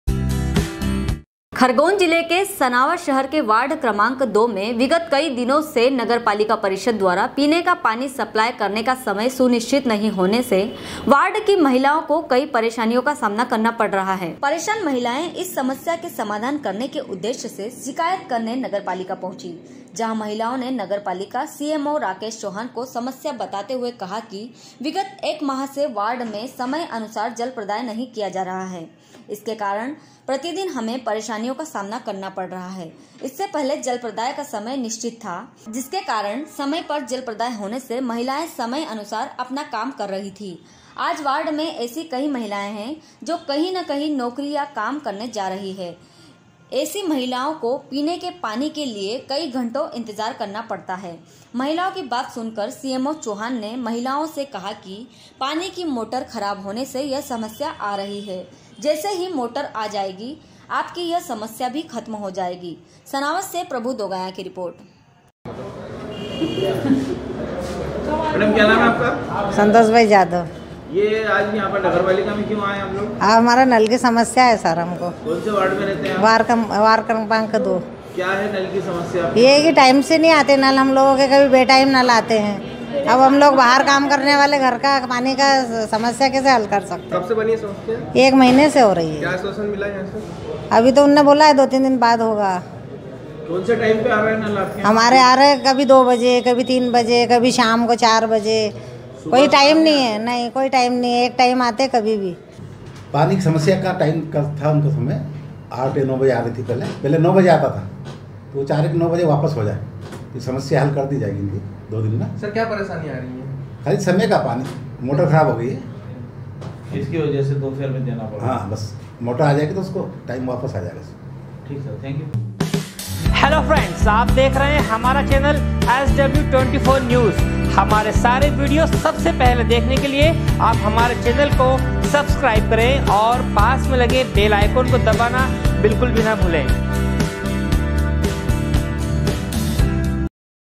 SW 24 News Madhyapradesh
खरगोन जिले के सनावद शहर के वार्ड क्रमांक 2 में विगत कई दिनों से नगर पालिका परीषद द्वारा पीने का पानी सप्लाय करने का समय सुनिशित नही होने से वार्ड की महिलाएं को कई परेशानीयो का सामना करना पड़ रहा है । सनावद से प्रभु दोगाया की रिर्पोट#sw